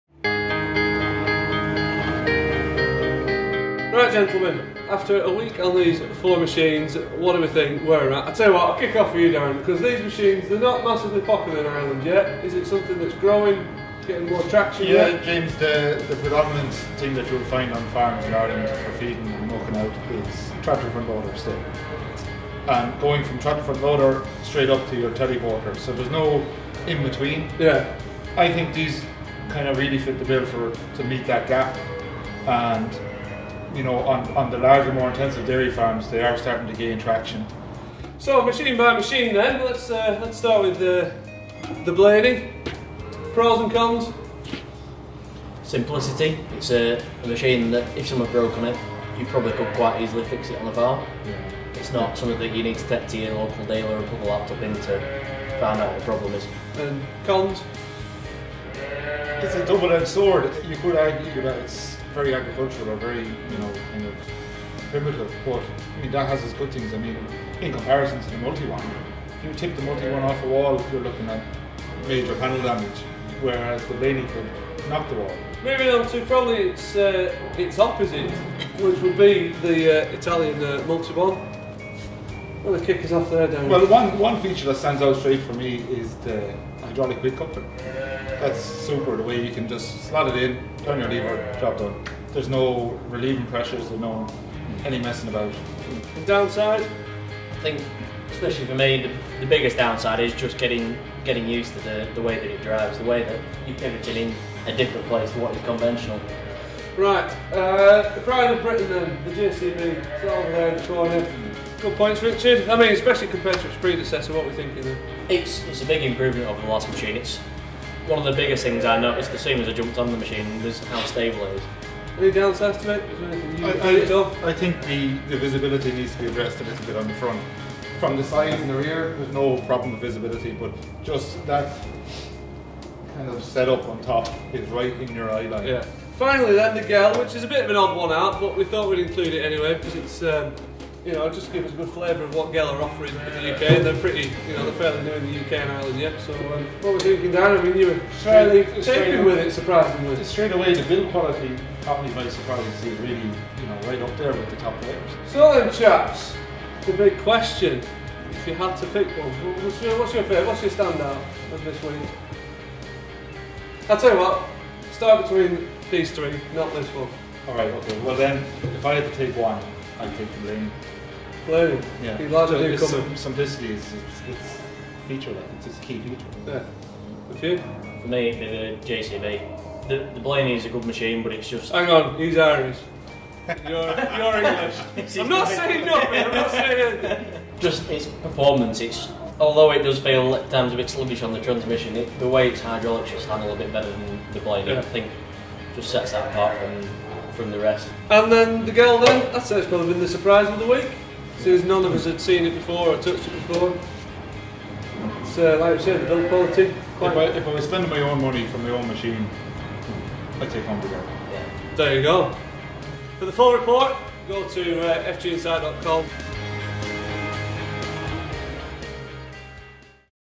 Mini loader comparison test: FG Verdict
While loaders continue to grow in capacity and physical size, unfortunately traditional farm buildings and those tight spots in the yard do not. Naturally, to fill this gap there has been a growing market for mini loaders, often to support larger loading machines on livestock farms.